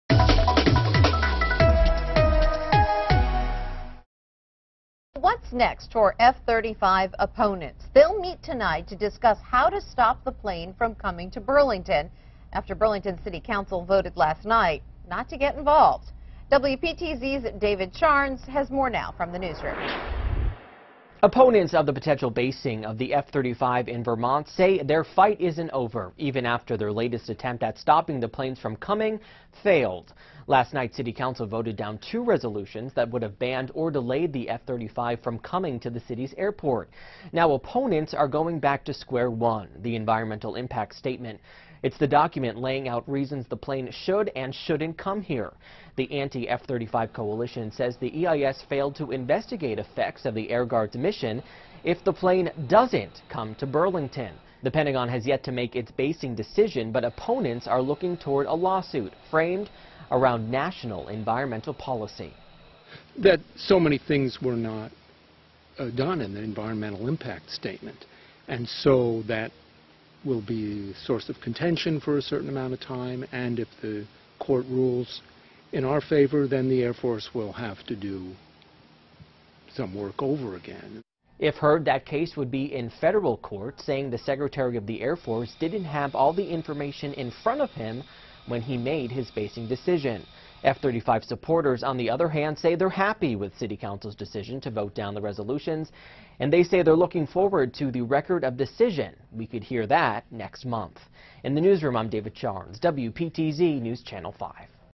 What's next for F-35 opponents?
Opponents of basing the F-35 in Vermont say they are unhappy with Monday's rejection from City Council, but are brainstorming ways to move forward.